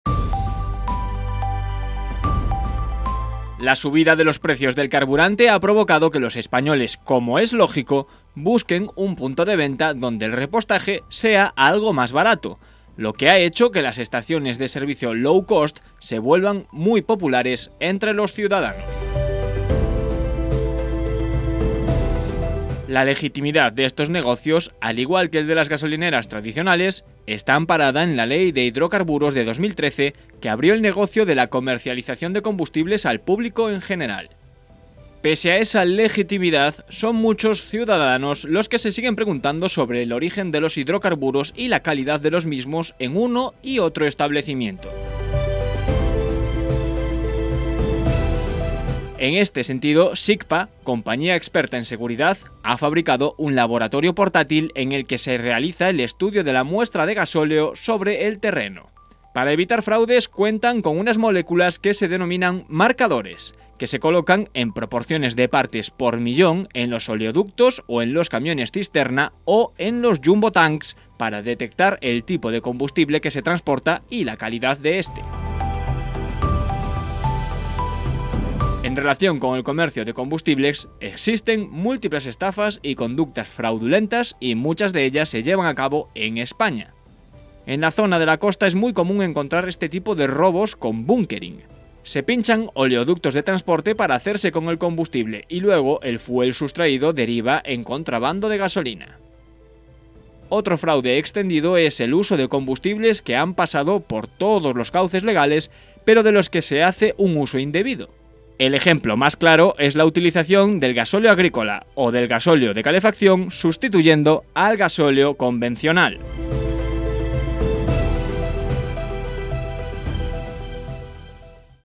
LAS ESTAFAS EN LOS COMBUSTIBLES | By CdRas
La subida de los precios del carburante ha provocado que los españoles, como es lógico, busquen un punto de venta donde el repostaje sea algo más barato, lo que ha hecho que las estaciones de servicio ‘low cost’ se vuelvan muy populares entre los ciudadanos.
La legitimidad de estos negocios, al igual que el de las gasolineras tradicionales, está amparada en la Ley de Hidrocarburos de 2013 que abrió el negocio de la comercialización de combustibles al público en general. Pese a esa legitimidad son muchos ciudadanos los que se siguen preguntando sobre el origen de los hidrocarburos y la calidad de los mismos en uno y otro establecimiento.
En este sentido, SICPA, compañía experta en seguridad, ha fabricado un laboratorio portátil en el que se realiza el estudio de la muestra de gasóleo sobre el terreno. Para evitar fraudes, cuentan con unas moléculas que se denominan “marcadores”, que se colocan en proporciones de partes por millón en los oleoductos o en los camiones cisterna o en los jumbo-tanks para detectar el tipo de combustible que se transporta y la calidad de este.
En relación con el comercio de combustibles existen múltiples estafas y conductas fraudulentas y muchas de ellas se llevan a cabo en España. En la zona de la costa es muy común encontrar este tipo de robos con el “bunkering. Se pinchan oleoductos de transporte para hacerse con el combustible y luego el fuel sustraído deriva en contrabando de gasolina. Otro fraude extendido es el uso de combustibles que han pasado por todos los cauces legales, pero de los que se hace un uso indebido. El ejemplo más claro es la utilización del gasóleo por el gasóleo convencional.